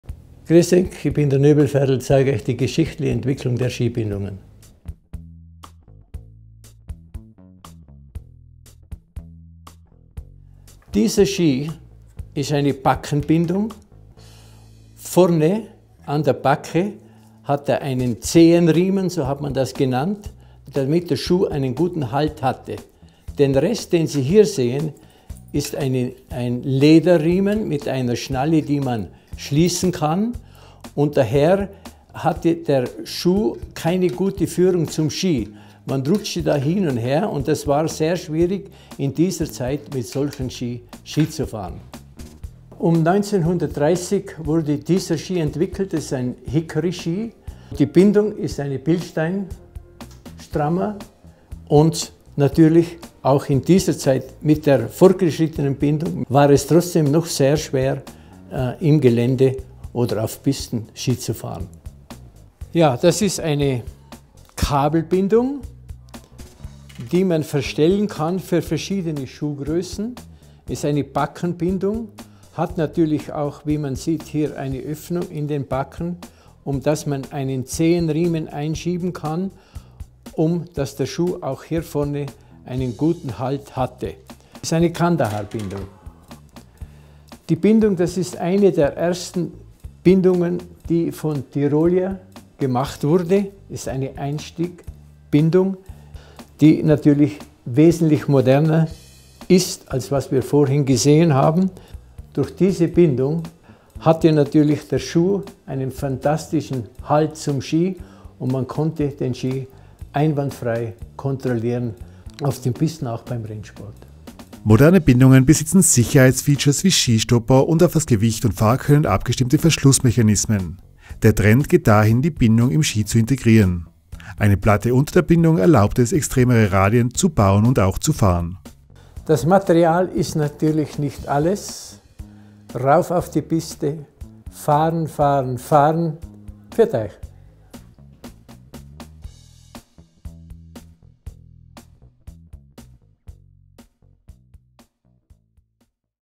Geschichte des Skilaufs: Bindungen
Ferdl Nöbl erzählt uns über die Entwicklung der Skibindung.